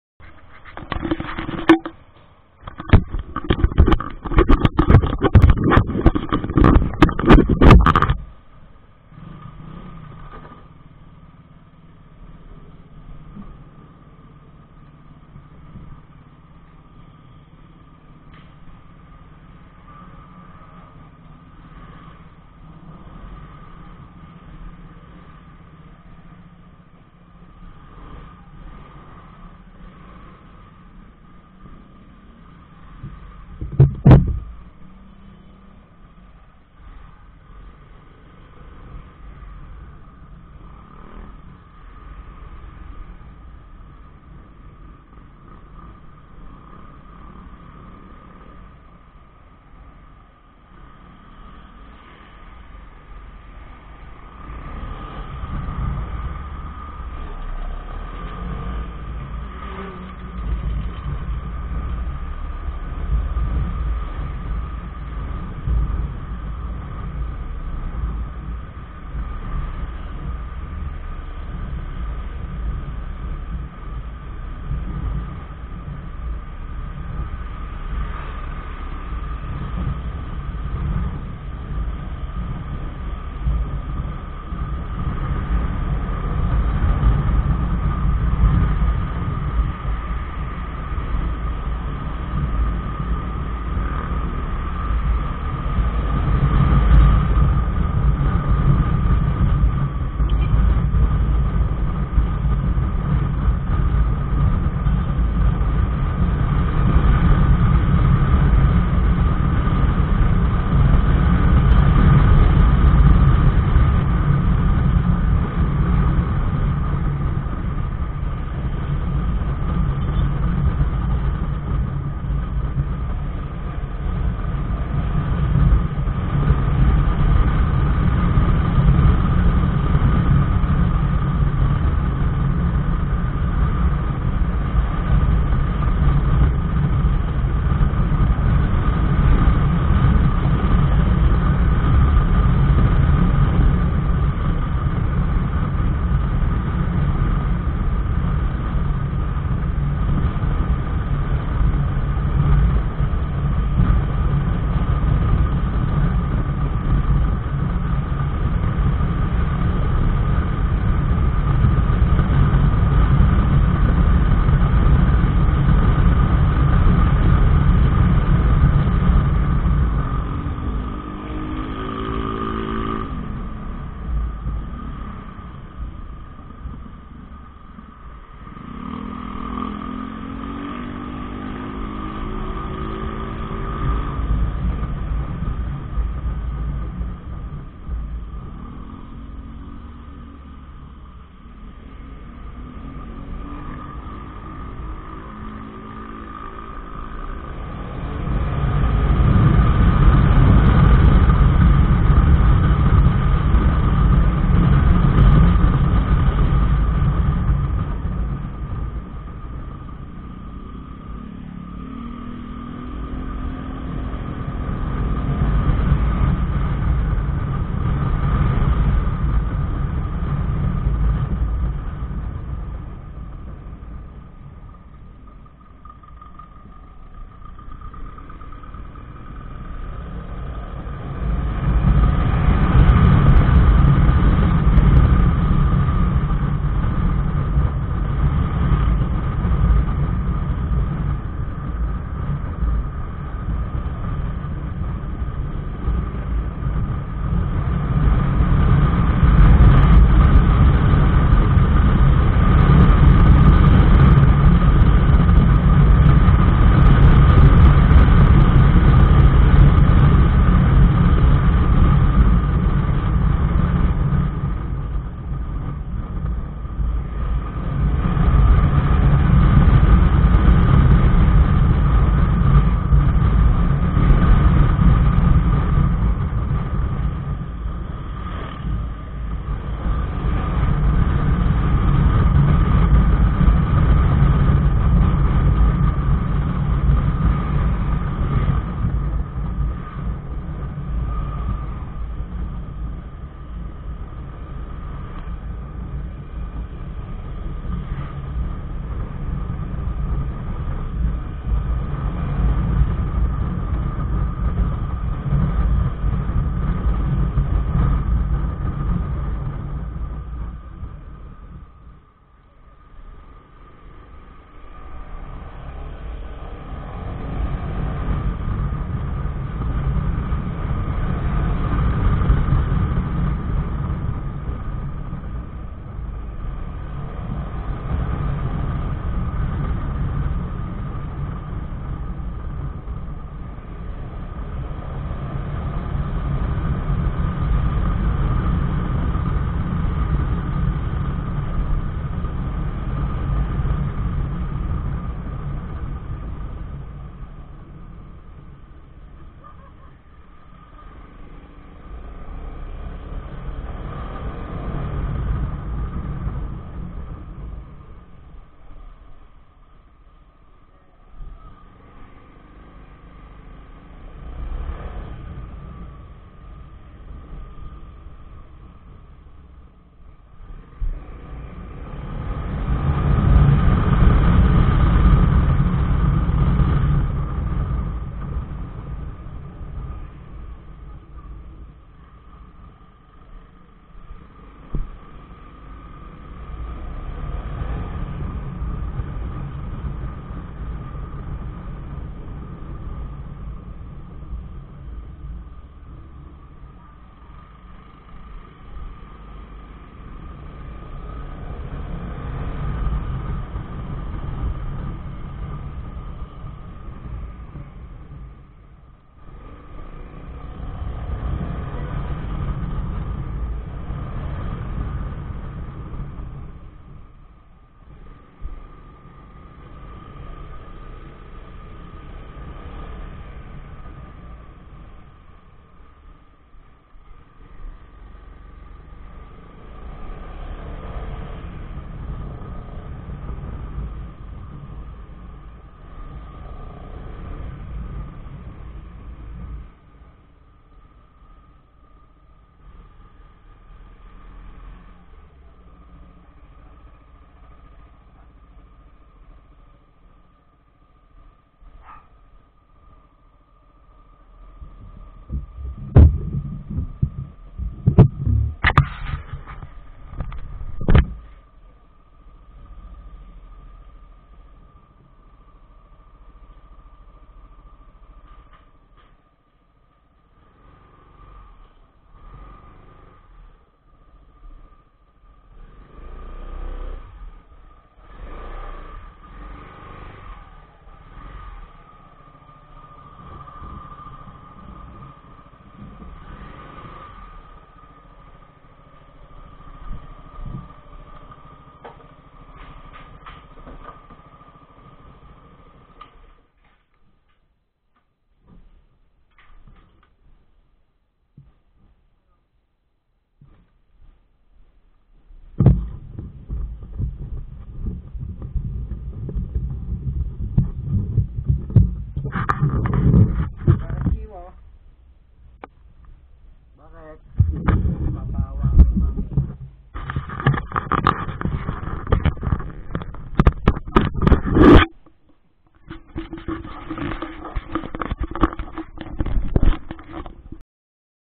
SJCam SJ4000 Wifi Action Camera night recording test..
Out going..
Testing SJCamera SJ400 at 720P 60fps at night time.
Subukan ko naman sa 1080P 60fps bukas.